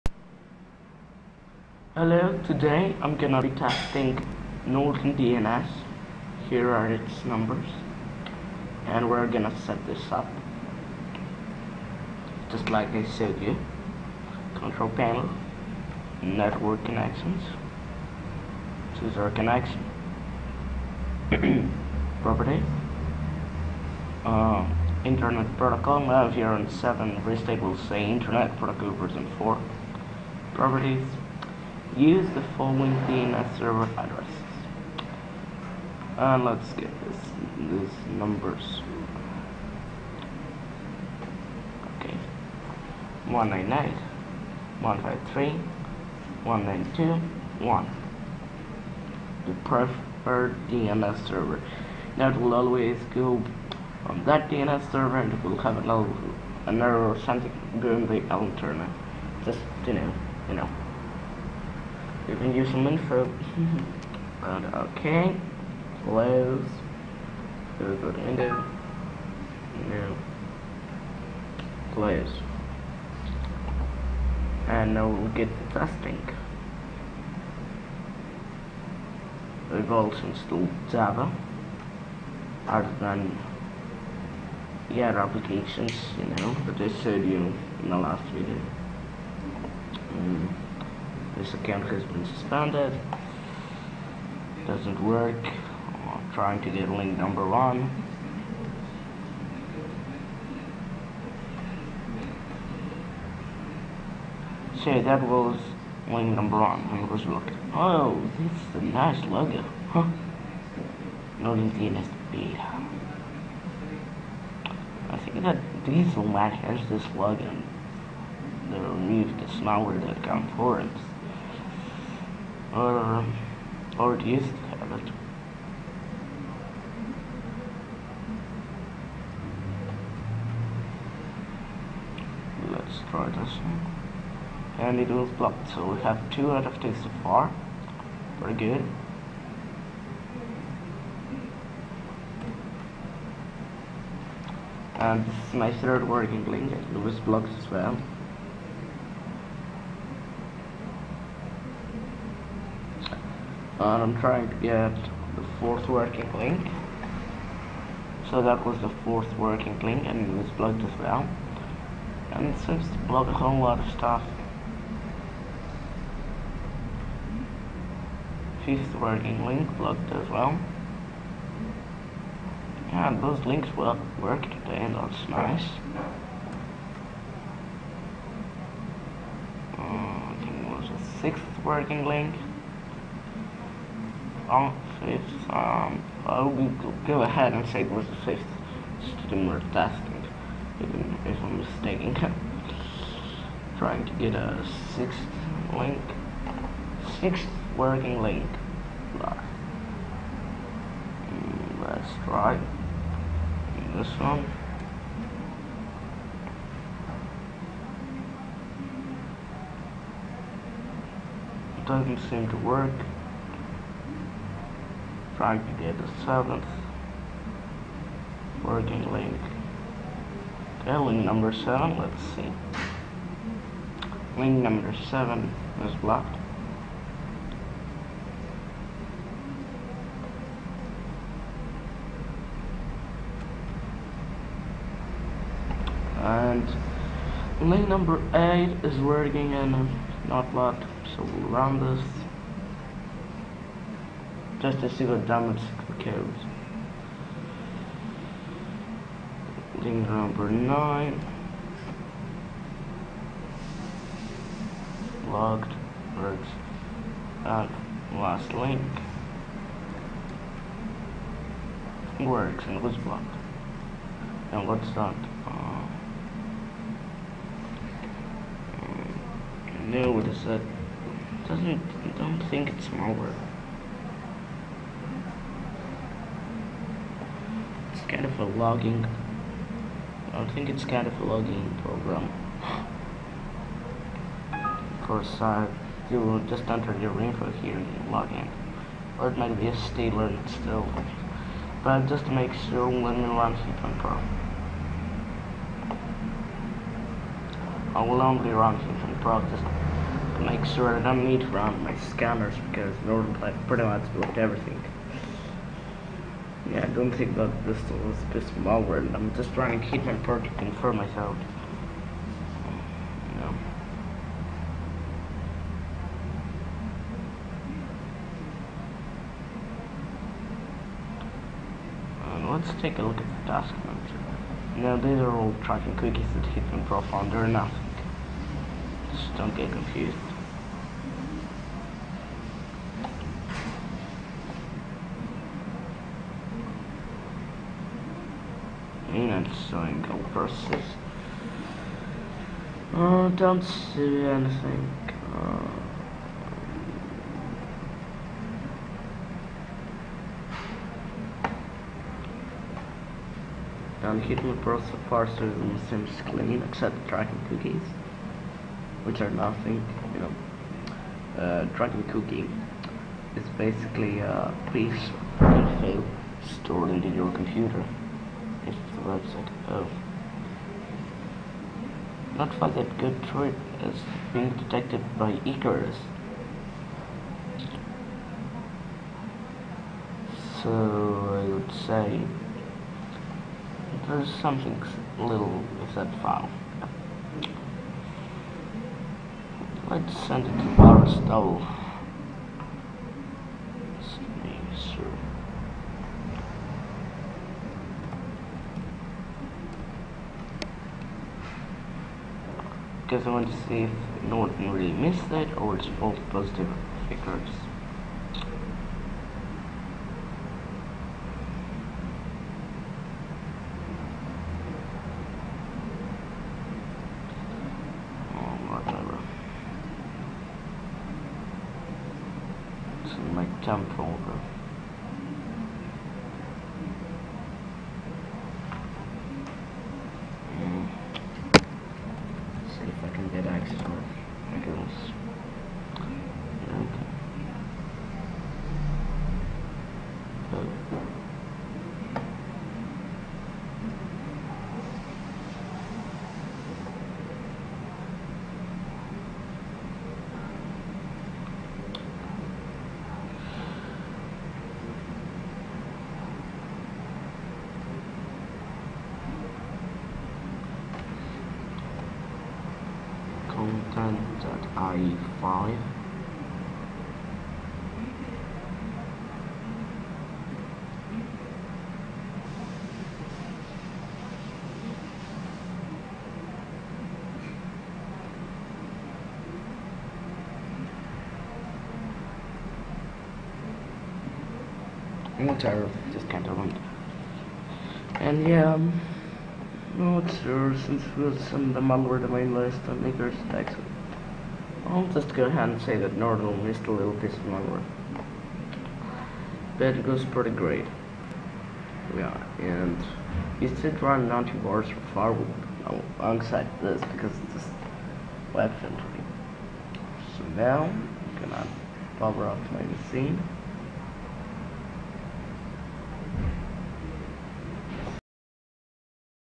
Norton DNS Prevention Test/Review
In this little video I test another free DNS service :)

NORTON DNS!
and as you can tell it's from Symantec (you know... the company that develops Norton AntiVirus and Norton Internet Security 2011)

Norton DNS was able to block around 90% of the malware and that was awesome for a dns service. You see, the dns service is just for web filtering and you should always have an antivirus alongside with it.